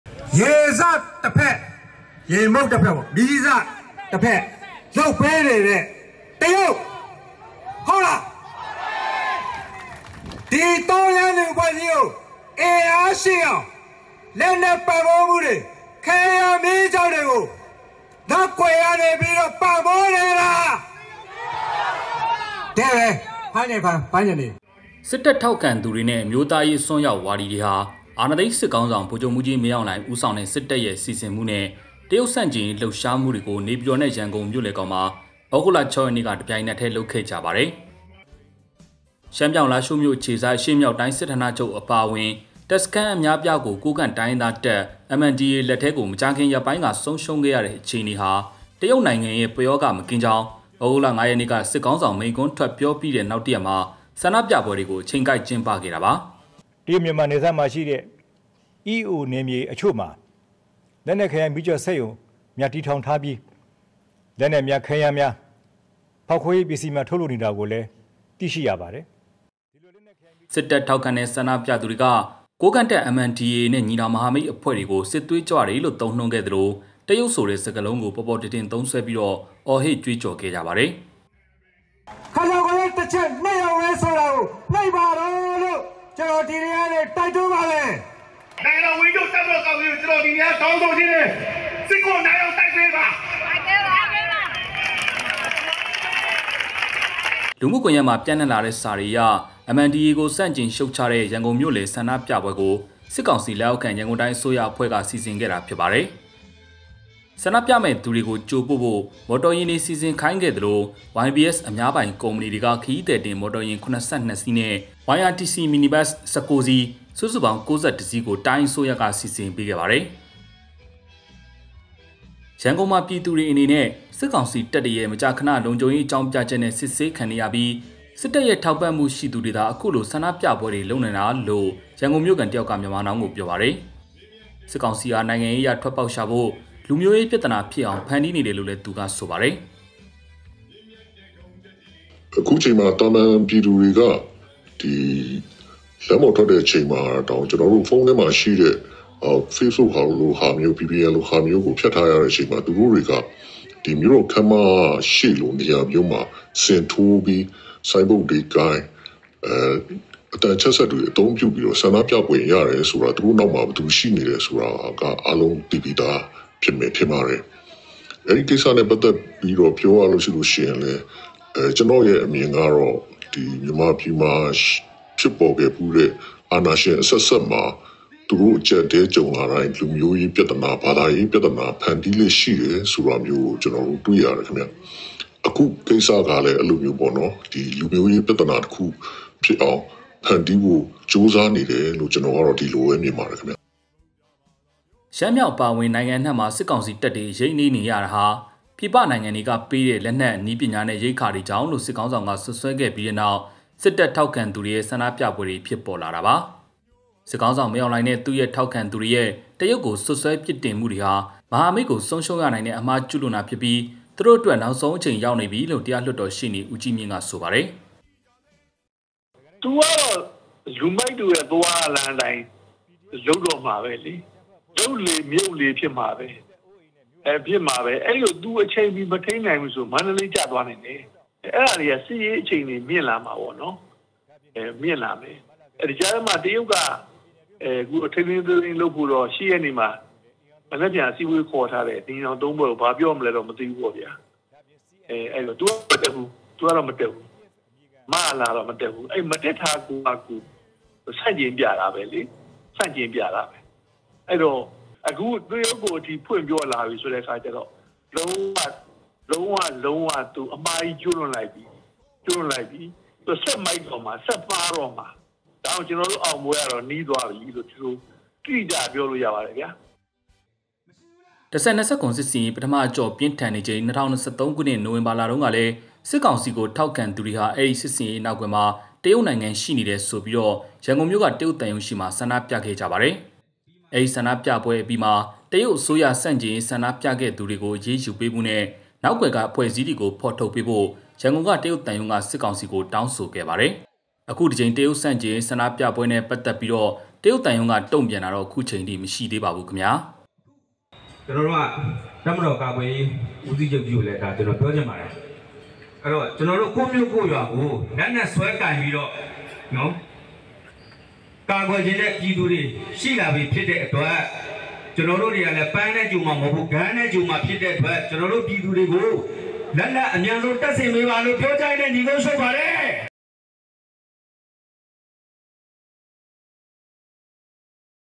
မင်းအောင်လှိုင်က တရုတ်ဆန့်ကျင်ရေးလှုပ်ရှားမှုများ ဆက်လုပ်ခိုင်း
တပ်ထောက်ခံသူတွေနဲ့ အမျိုးသားရေးအစွန်းရောက်ဝါဒီတွေဟာ အာဏာသိမ်းစစ်ခေါင်းဆောင် ဗိုလ်ချုပ်မှူးကြီးမင်းအောင်လှိုင် ဦးဆောင်တဲ့ စစ်တပ်ရဲ့စီစဉ်မှုနဲ့ တရုတ်ဆန့်ကျင်ရေးလှုပ်ရှားမှုတွေကို နေပြည်တော်နဲ့ ရန်ကုန်မြို့လယ်ခေါင်မှာ ဩဂုတ်လ ၆ရက်နေ့က တပြိုင်ထဲ လုပ်ခဲ့ကြပါတယ်။

ဆန္ဒပြသူတွေက 'တရုတ်' ဆိုတဲ့ စကားလုံးကို ပေါ်ပေါ်ထင်ထင်သုံးစွဲပြီး အော်ဟစ်ကြွေးကြော်ခဲ့ကြပါတယ်။

ရှမ်းမြောက် လားရှိုးမြို့ အခြေစိုက် အရှေ့မြောက်တိုင်းစစ်ဌာနချုပ် အပါအဝင် တပ်စခန်းအများအပြားကို ကိုးကန့်တိုင်းရင်းသားတပ် MNDDA လက်ထဲ မကြာခင်ရက်ပိုင်းက ဆုံးရှုံးခဲ့ရတဲ့  အခြေအနေဟာ  တရုတ်နိုင်ငံရဲ့  ပယောဂမကင်းကြောင်း သြဂုတ်လ ၅ ရက်နေ့က စစ်ခေါင်းဆောင် မိန့်ခွန်းထွက်ပြောပြီးတဲ့ နောက်တစ်ရက်မှာ ဆန္ဒပြပွဲတွေကို အချိန်ကိုက် ကျင်းပခဲ့တာပါ။

“၁၀၂၇” စစ်ဆင်ရေး ပထမအကျော့ပြင်းထန်နေချိန် ၂၀၂၃  နိုဝင်ဘာလတုန်းကလည်း စစ်ကောင်စီကို ထောက်ခံသူတွေဟာ အဲ့ဒီစစ်ဆင်ရေးနောက်ကွယ်မှာ တရုတ်နိုင်ငံရှိနေတယ်ဆိုပြီး ရန်ကုန်မြို့ တရုတ်သံရုံးရှေ့ မှာ ဆန္ဒပြခဲ့ကြပါတယ်။

သတင်း၊ တင်ဆက် - မြတ်ကြီး